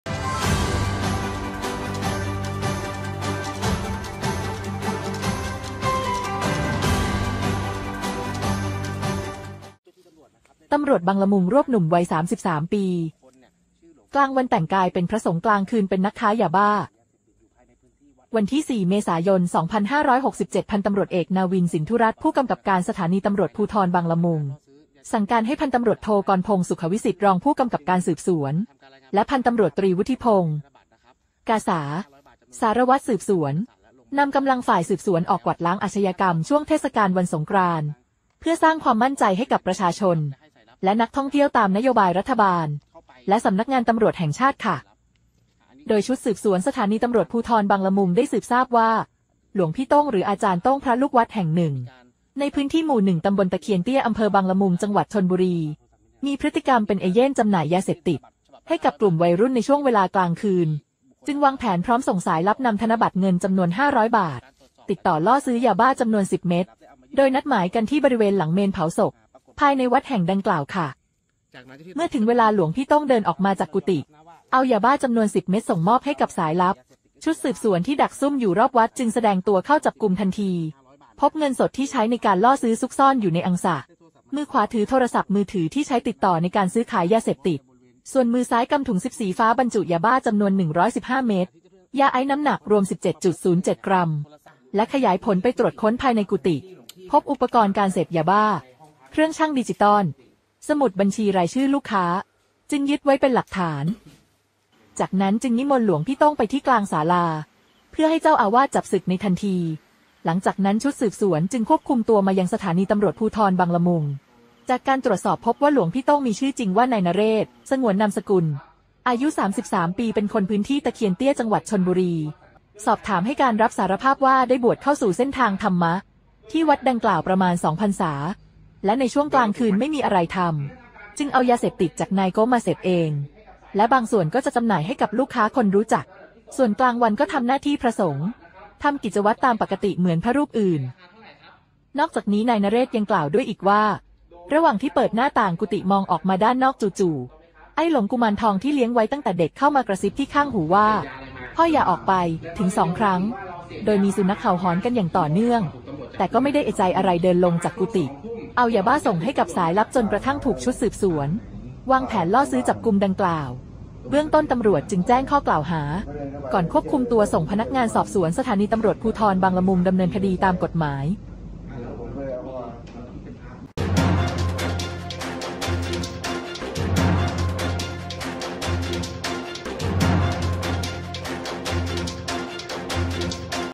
ตำรวจบางละมุง รวบหลวงพี่โต้ง คาเมรุวัด ลอบค้ายานรก เผย ไอ้หลง กุมารทอง กระซิบข้างหู “พ่ออย่าออกไป”
ตำรวจบางละมุง รวบหลวงพี่โต้ง คาเมรุวัด ลอบค้ายานรก เผย ไอ้หลง กุมารทอง กระซิบข้างหู “พ่ออย่าออกไป” แต่ไม่ฟัง!!

     (4 เม.ย.) พ.ต.อ.นาวิน สินธุรัตน์ ผกก.สภ.บางละมุง สั่งการให้ พ.ต.ท.กรณ์พงษ์ สุขวิสิฏฐ์ รอง ผกก.สส.ฯ และ พ.ต.ต.วุฒิพงษ์ กาสา สว.สส.ฯ นำกำลังฝ่ายสืบสวน ออกกวาดล้างอาชญากรรมช่วงเทศกาลวันสงกรานต์ เพื่อสร้างความมั่นใจให้กับประชาชน และนักท่องเที่ยว ตามนโยบายรัฐบาล และสำนักงานตำรวจแห่งชาติ

โดยชุดสืบสวน สภ.บางละมุง ได้สืบทราบว่า หลวงพี่โต้ง หรืออาจารย์โต้ง พระลูกวัดแห่งหนึ่ง ในพื้นที่ ม.1 ต.ตะเคียนเตี้ย อ.บางละมุง จ.ชลบุรี มีพฤติกรรมเป็นเอเย่นต์จำหน่ายยาเสพติดให้กับกลุ่มวัยรุ่นในช่วงเวลากลางคืน จึงวางแผนพร้อมส่งสายลับนำธนบัตรเงินจำนวน 500 บาท ติดต่อล่อซื้อยาบ้าจำนวน 10 เม็ด นัดหมายกันที่บริเวณหลังเมรุเผาศพ ภายในวัดแห่งดังกล่าว

เมื่อถึงเวลา พระขันติจิตโต หรือหลวงพี่โต้ง เดินออกมาจากกุฎิ เอายาบ้าจำนวน 10 เม็ด ส่งมอบให้กับสายลับ หลังความผิดสำเร็จ ชุดสืบสวนที่ดักซุ่มอยู่รอบวัดจึงแสดงตัวเข้าจับกุมทันที พบเงินสดที่ใช้ในการล่อซื้อซุกซ่อนอยู่ในอังสะ มือขวาถือที่ใช้ติดต่อในการซื้อขายยาเสพติด ส่วนมือซ้ายกำถุงซิปสีฟ้าบรรจุ ยาบ้าจำนวน 115 เม็ด ยาไอซ์ น้ำหนักรวม 17.07 กรัม ก่อนขยายผลไปตรวจค้นภายในกุฎิ พบอุปกรณ์การเสพยาบ้า เครื่องชั่งดิจิตอล สมุดบัญชีรายชื่อลูกค้า จึงยึดไว้เป็นหลักฐาน

จากนั้นจึงนิมนต์ พระขันติจิตโต หรือหลวงพี่โต้ง ไปที่กลางศาลาเพื่อให้เจ้าอาวาสจับสึกในทันที ก่อนชุดสืบสวนจึงควบคุมตัวมายัง สภ.บางละมุง จากการตรวจสอบพบว่า หลวงพี่โต้ง มีชื่อจริงว่า นายนเรศ (สงวนนามสกุล) อายุ 33 ปี เป็นคนพื้นที่ตะเคียนเตี้ย จ.ชลบุรี สอบถามให้การรับสารภาพว่า ได้บวชเข้าสู่เส้นทางธรรมะที่วัดดังกล่าวประมาณ 2 พรรษา และในช่วงกลางคืนไม่มีอะไรทำจึงเอายาเสพติดจากนายโก๊ะ มาเสพเองและบางส่วนก็จะจำหน่ายให้กับลูกค้าคนรู้จัก ส่วนกลางวันก็ทำหน้าที่พระสงฆ์ตามปกติ เหมือนพระรูปอื่น

นอกจากนี้ นายนเรศ ยังกล่าวด้วยอีกว่า ระหว่างที่เปิดหน้าต่างกุฎิ มองออกมาด้านนอก จู่ๆ ไอ้หลง “กุมารทอง” ที่เลี้ยงไว้ตั้งแต่เด็กเข้ามากระซิบที่ข้างหูว่า “พ่ออย่าออกไป” ถึง 2 ครั้ง โดยมีสุนัขเห่าหอนกันอย่างต่อเนื่อง แต่ก็ไม่ได้เอะใจอะไร เดินลงจาก กุฎิเอายาบ้าส่งให้กับสายลับ จนกระทั่งถูกชุดสืบสวนวางแผนล่อซื้อจับกุมดังกล่าว เบื้องต้นตำรวจจึงแจ้งข้อกล่าวหา ก่อนควบคุมตัวส่งพนักงานสอบสวน สภ.บางละมุง ดำเนินคดีตามกฏหมาย.